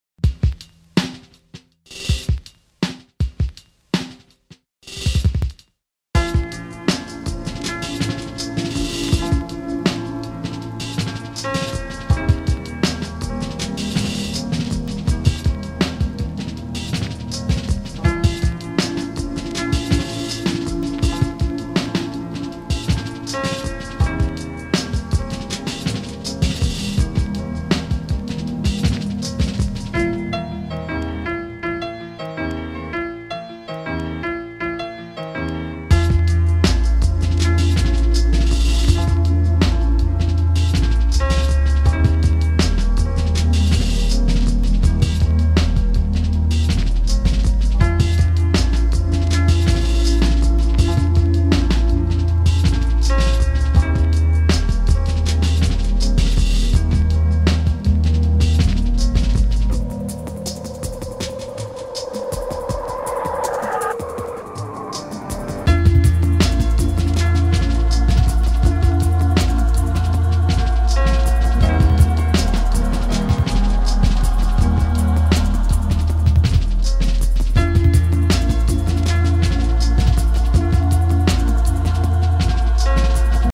a short ride at the locals
a short ride at the locals with one of my friends